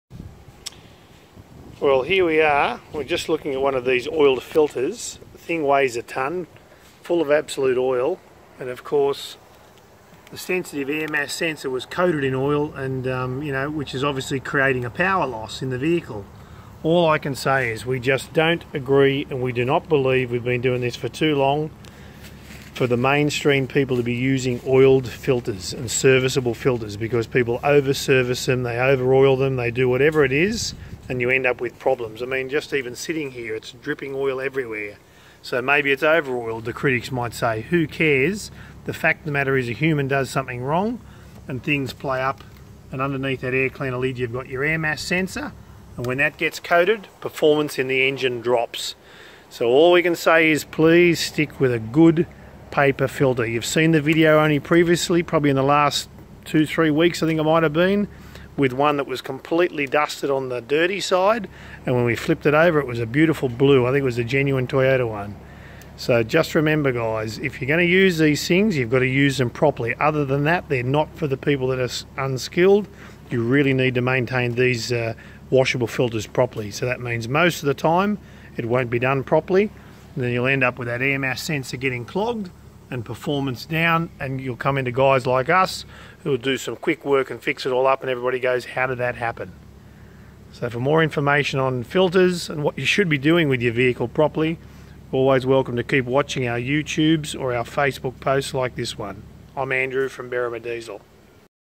Oiled Air filters and Power Loss
Friday Bad Foam Hair Day.

Time and time again we see problems caused by oiled air filters. See what we see and gain some engine power while you're at it.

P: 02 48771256
Or PMessage us right here on Facebook